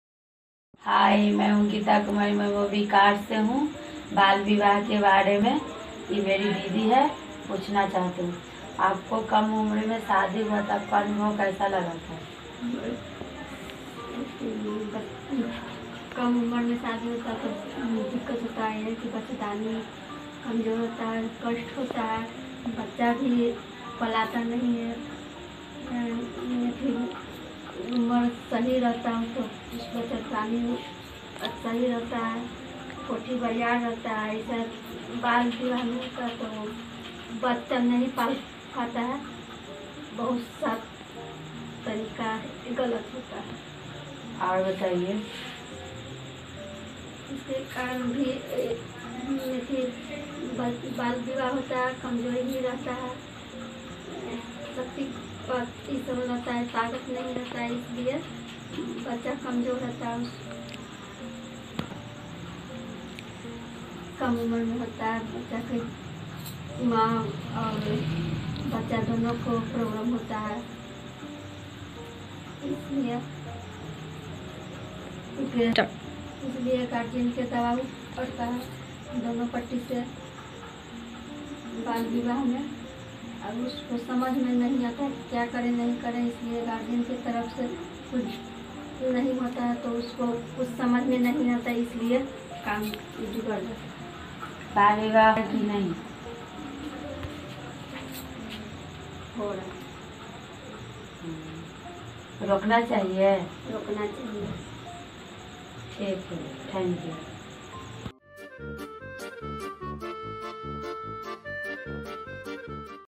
Mobikaar Gita’s interview on Marriage at an early age | Bihar
Trainees of the Love Matters India's Mobikaar public interest journalism programme in Bihar interviewed a member of their community about their experience of marriage at an early age. In their video, Mobikaar Gita speaks to a woman who says “Child marriage should be stopped.“

लव मैटर्स इंडिया के मोबिकार (जनहित पत्रकारिता कार्यक्रम) के बिहार के प्रतिभागियों ने अपने समुदाय के एक सदस्य से उनके कम उम्र में शादी के अनुभव के बारे में बात की। अपने वीडियो में, मोबिकार गीता एक महिला से बात करती है जो कहती हैं "बाल विवाह को रोकना चाहिए।”